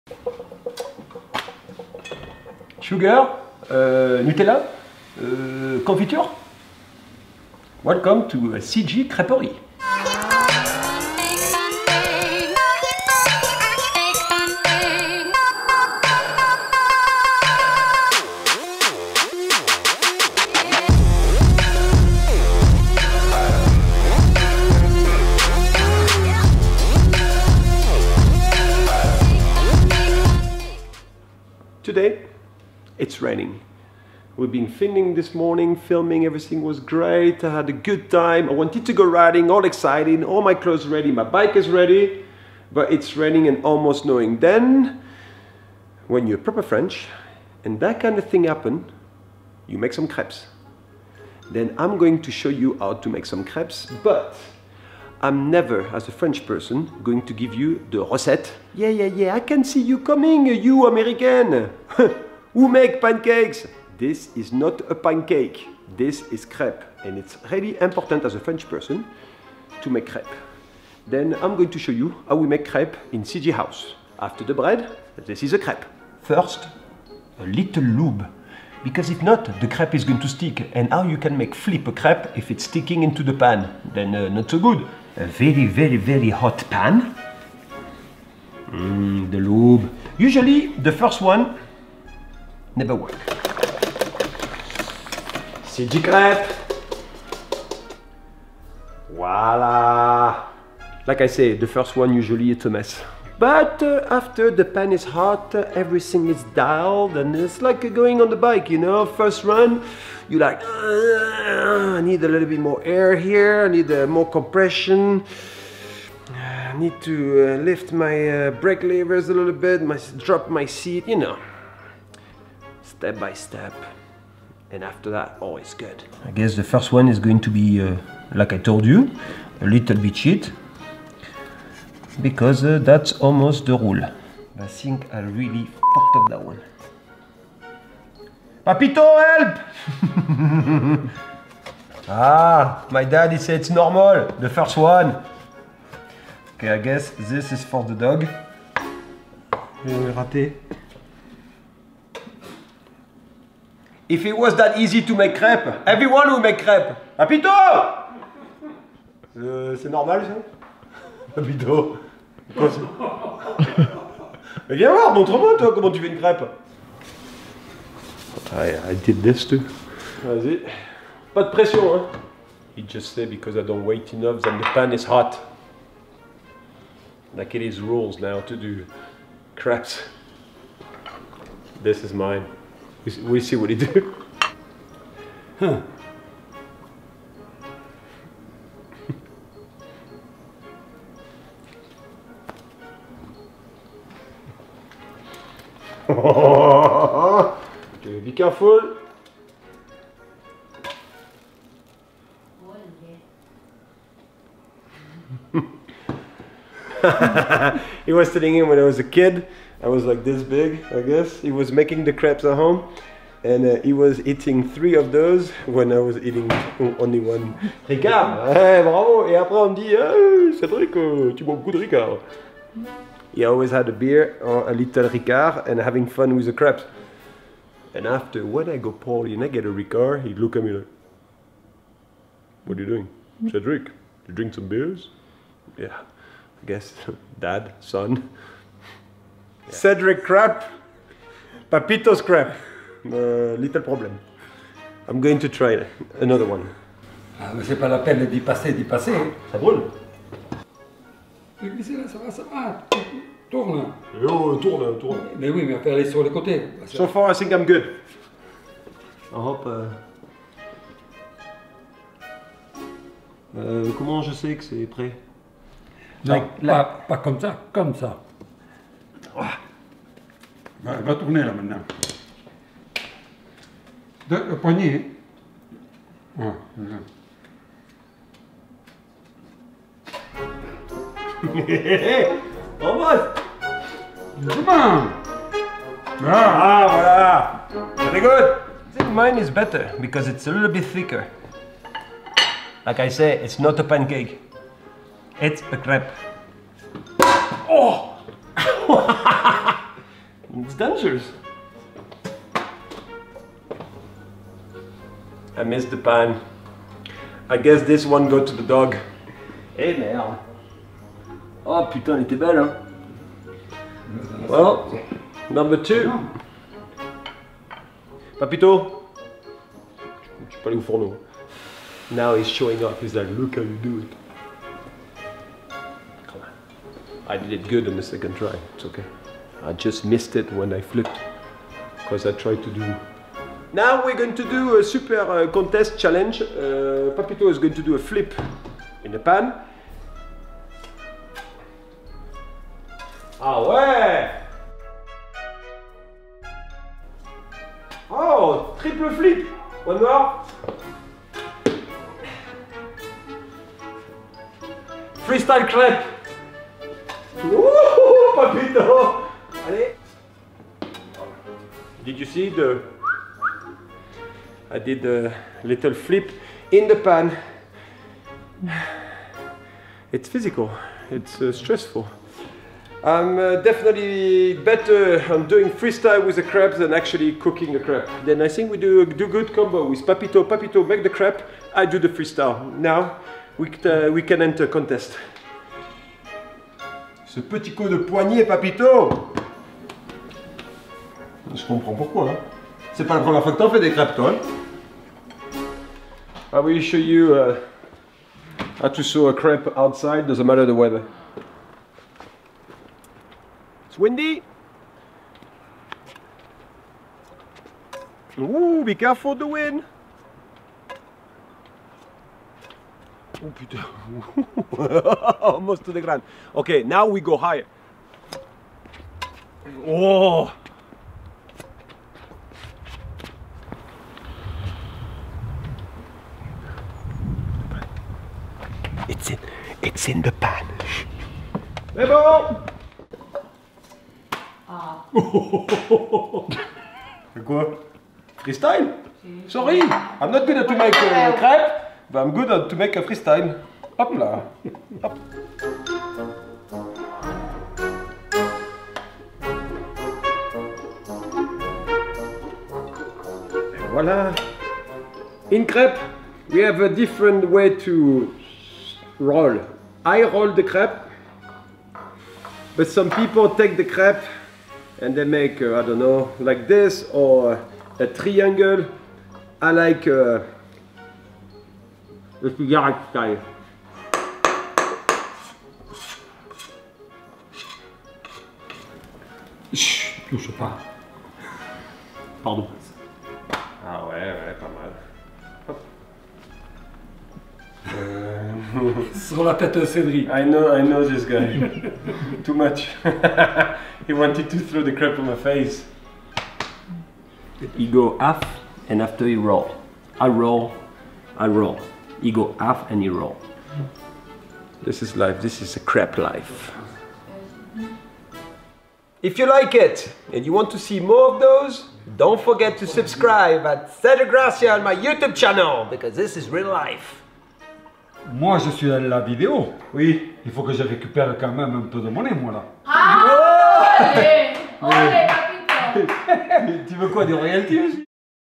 HAPPY PANCAKE DAY 2017 With PAPITO The Crêpe MASTER - CG VLOG #83
Happy crêpe* day ! I thought I could cook ... Luckily Papito came to help me out ! Glad I'm doing ok with the freestyle part !

A new awesome video is uploaded everyday to follow my adventures around the world as a pro mountain biker for over 20 years.